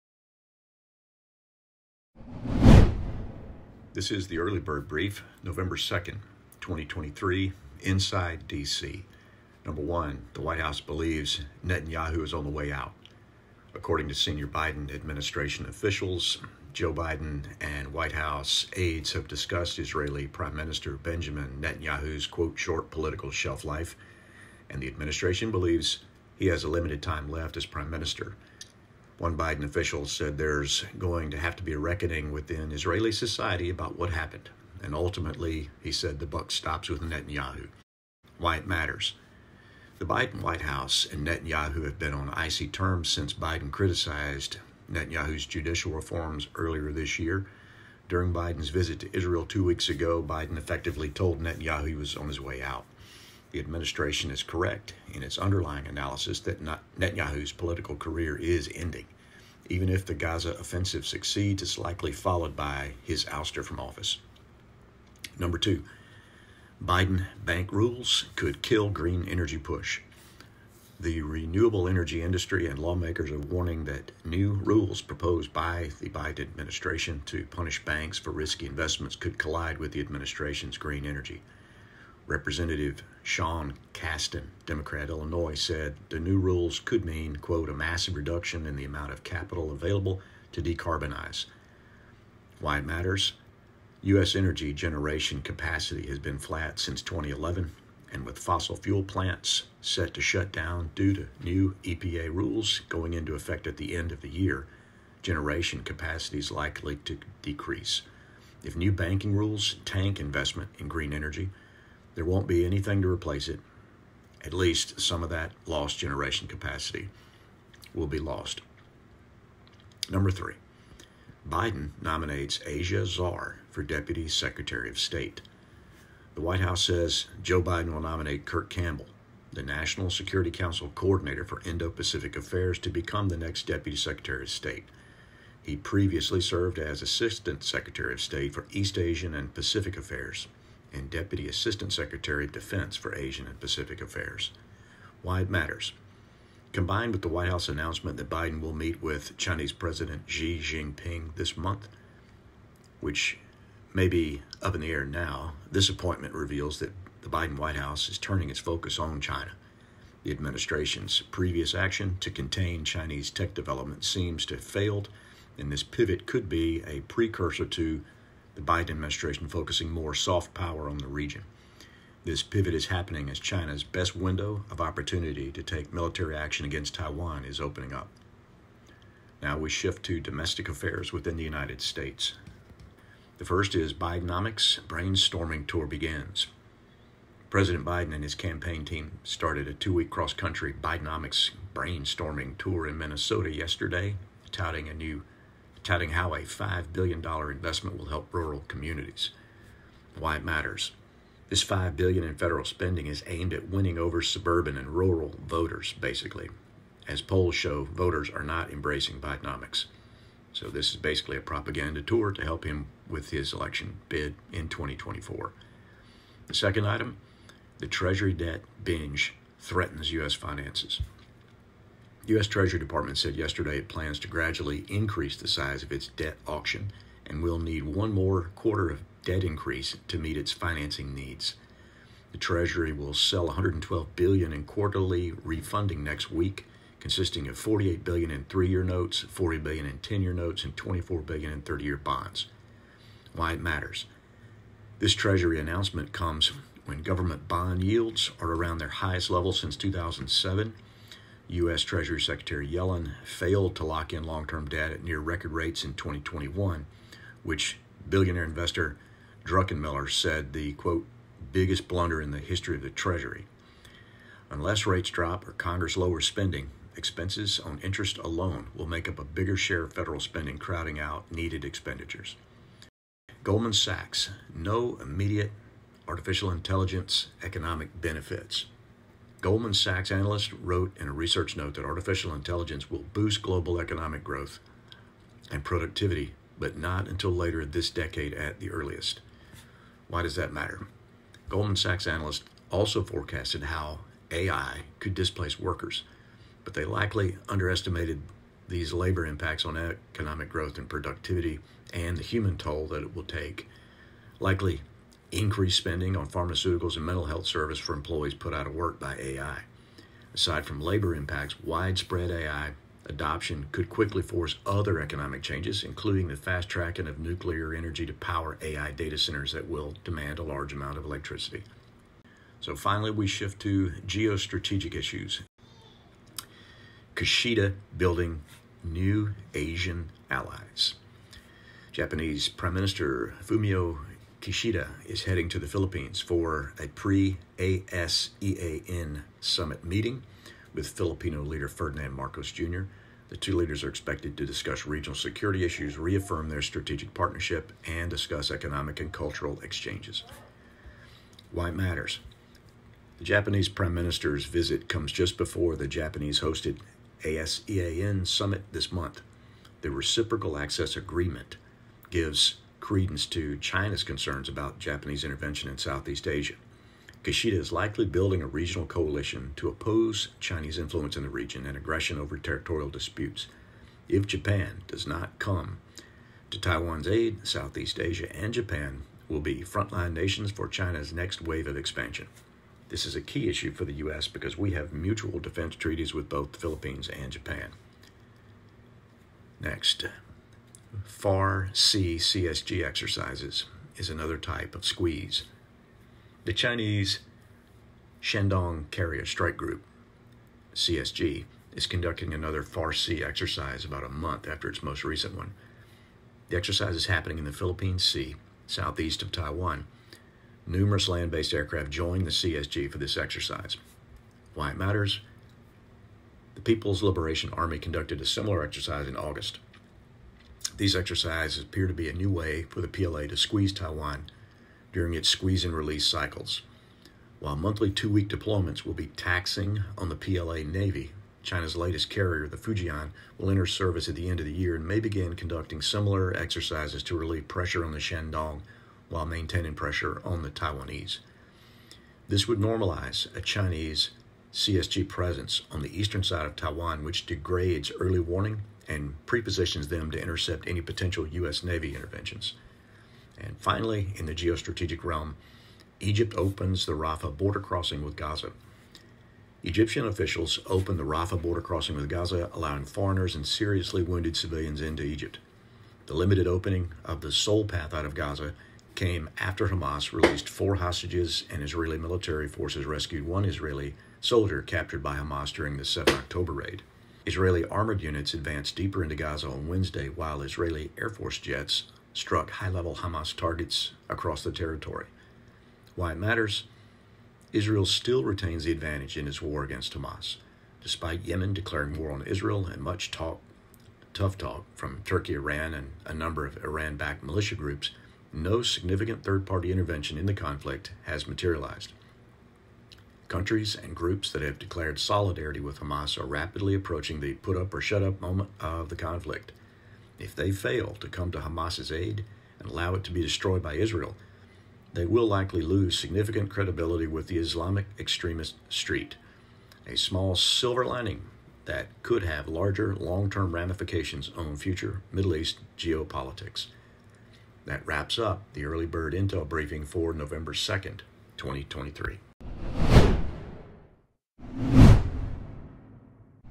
The Early Bird Intelligence Brief (02NOV2023)
This is an abbreviated audio version of the Early Bird Intelligence Brief covering three major areas for the date of 02 November 2023.

To get the full version of the Early Bird visit -